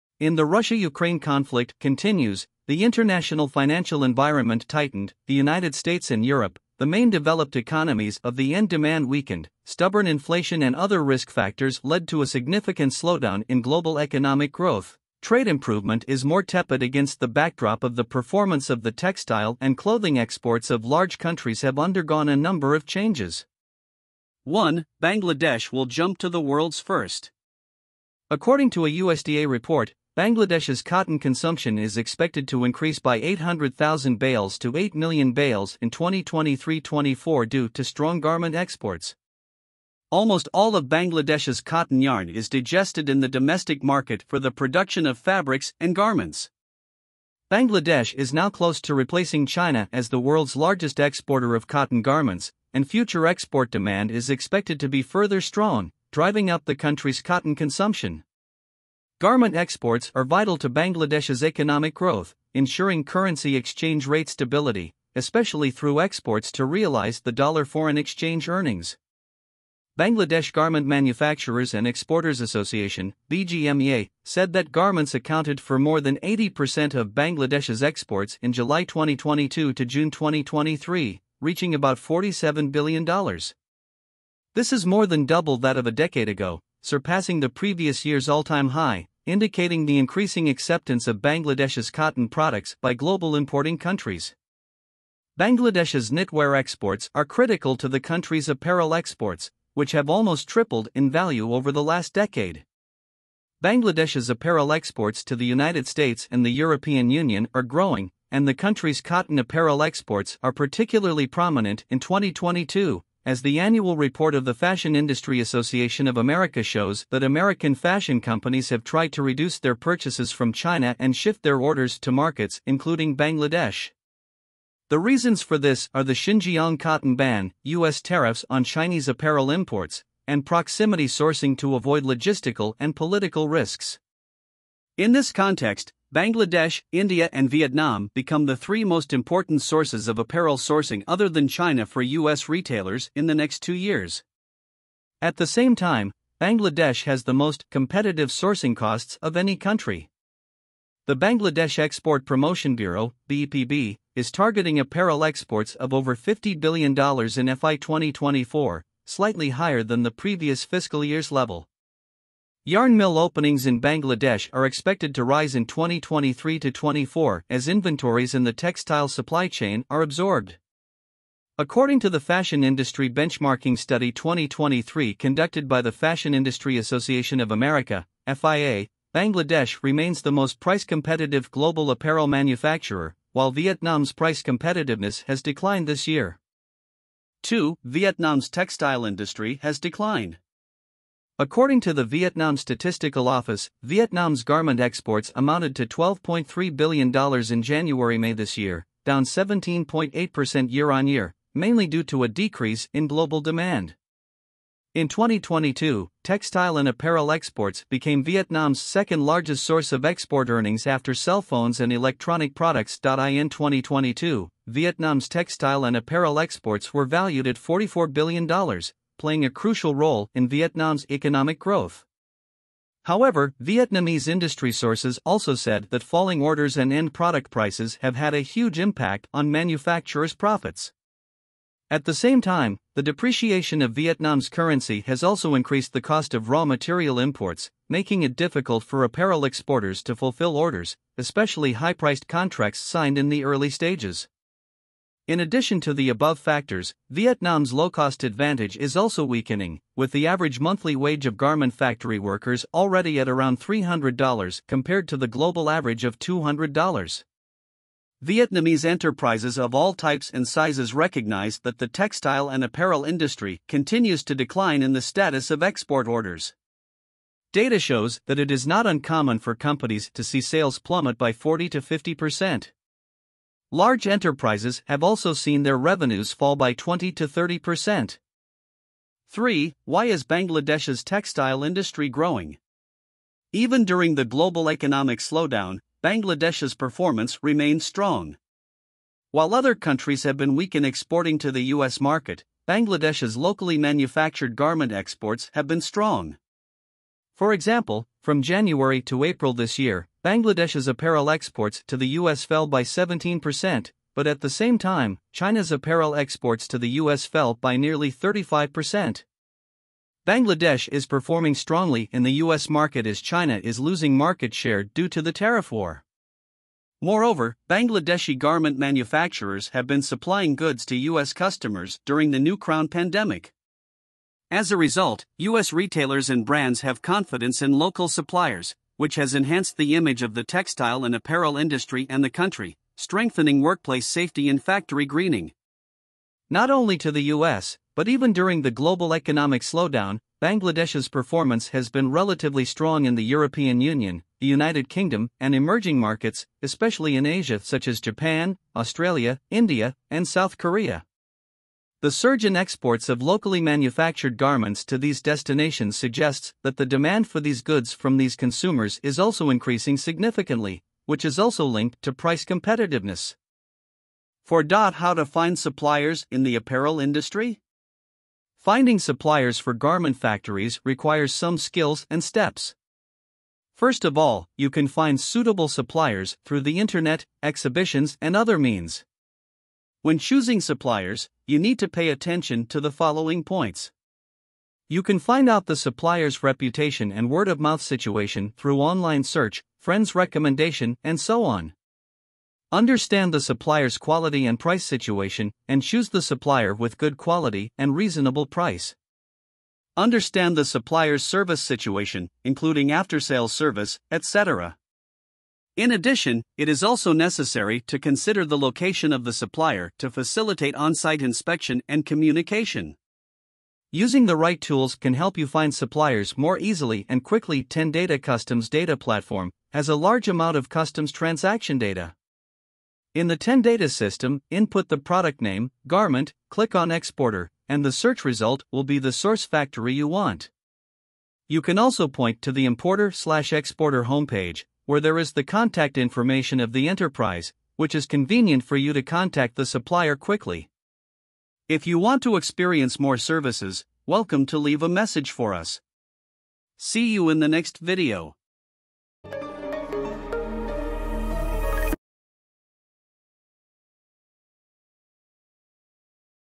U.S. Garment Import Orders Plunge 30% ｜Will Bangladesh Surpass China in Garment Exports?
In the Russia-Ukraine conflict continues, the international financial environment tightened, the United States and Europe, the main developed economies of the end demand weakened, stubborn inflation and other risk factors led to a significant slowdown in global economic growth, trade improvement is more tepid against the backdrop of the performance of the textile and clothing exports of large countries have undergone a number of changes. Watch this video to get more understanding about garment trading.

Benefits of using Tendata for importers and exporters.